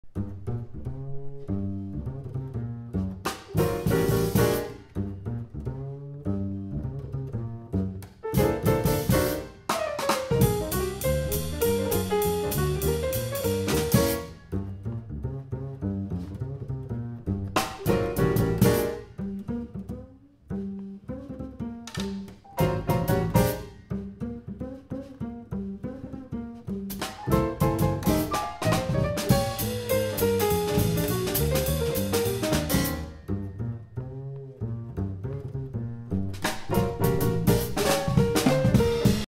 When the bass player is in charge
Playing with the Brandon Robertson Trio at the Bonita Center for the Performing Arts in FL.